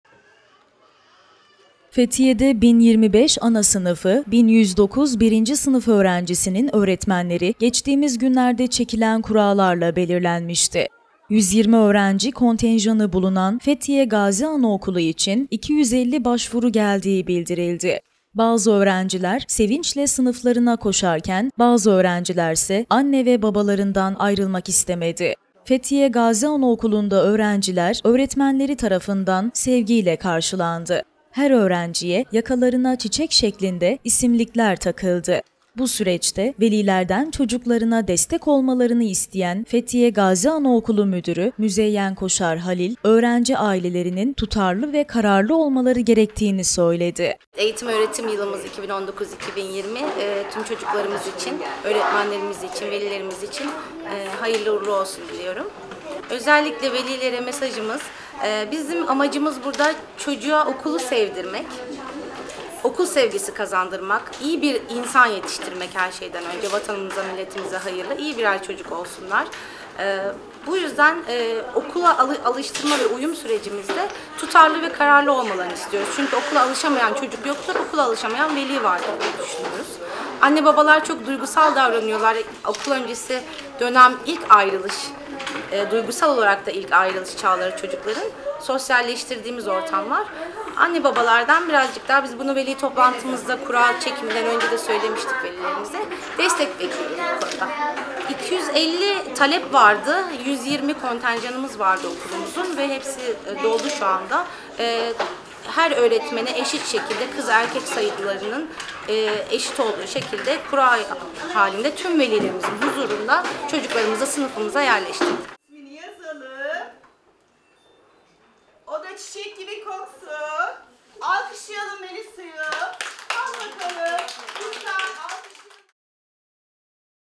ANASINIFI ÖĞRENCİLERİ UYUM PROGRAMINDA
Okula adaptasyon süreci için anasınıfı ve 1. sınıflara uygulanan uyum programınaFethiye’de de başlandı. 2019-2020 Eğitim Öğretim Yılıbaşlamadan hemen önce anasınıfı ve 1. sınıfta   eğitim alan öğrenciler, uyum haftası kapsamında okula iki gün erken başladılar.